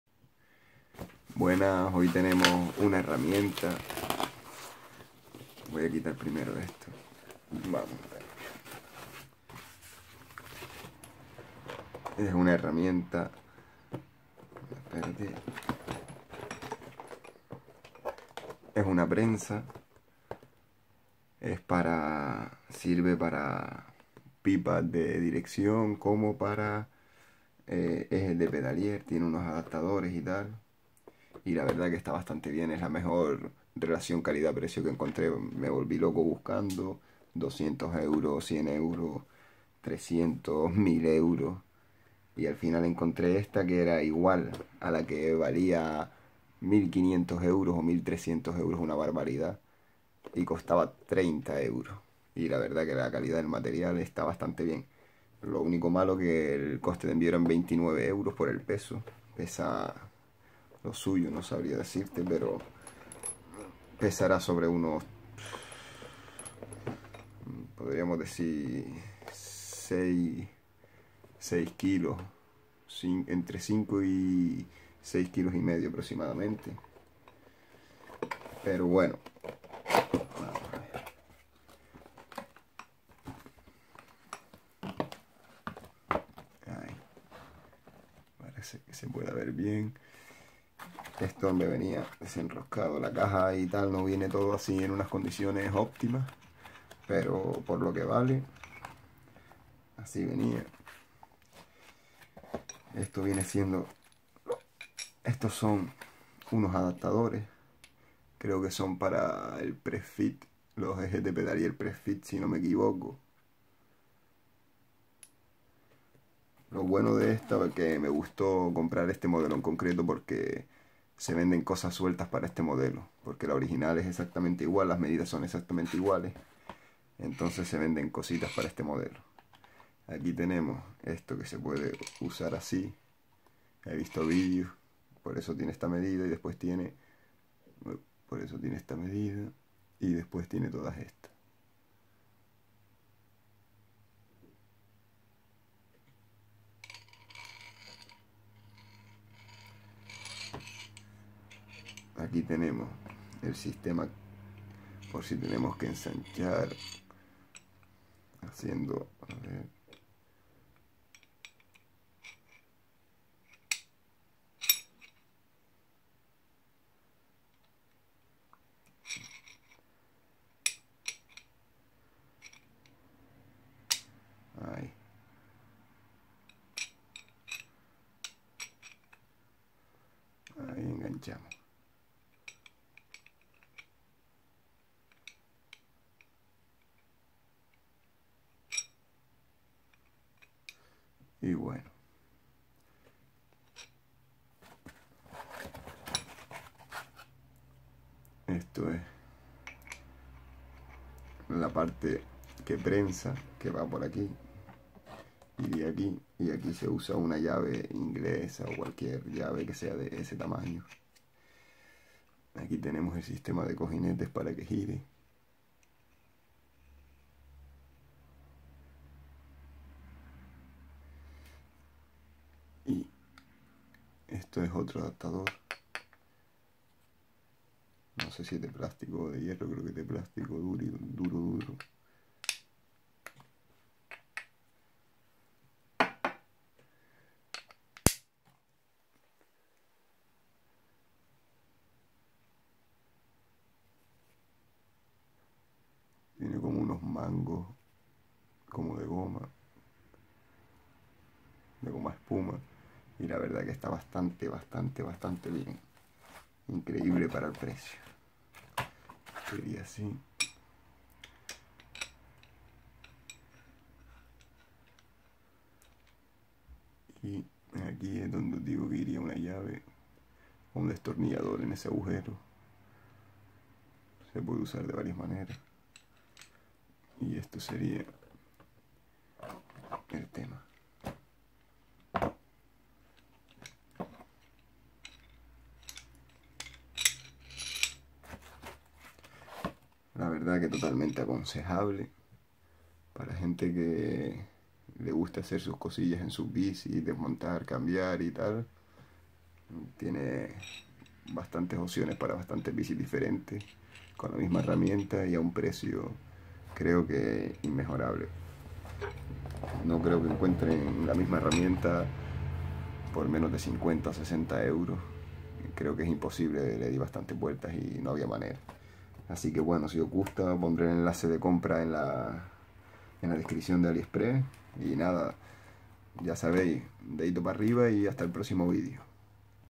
HERRAMIENTAS MTB Prensa para Direccion y Pressfit Aliexpress
Buenas hoy os enseño una herramienta para montar los rodamientos en la pipa de direccion y tambien para los ejes de Pedaliers del tipo Pressfit en bastantes variantes diferentes la verdad que relacion calidad/precio (60€ incluido gastos de envio)no encontre nada igual, la siguiente opcion que tenia costaba 200€ y no era ese modelo, estaba mucho mas limitada en cuanto a diferentes medidas de direcciones y ejes de pedaliers, a continuacion dejo el enlace de compra:
Durable Taiwan 1 "1-1/8" 1-1/2 "Mountain Road MTB BMX Bike Headset Bicicletas de Prensa En Herramienta de Acero CP Bottome Soporte De Prensa En la Herramienta
(from AliExpress Android)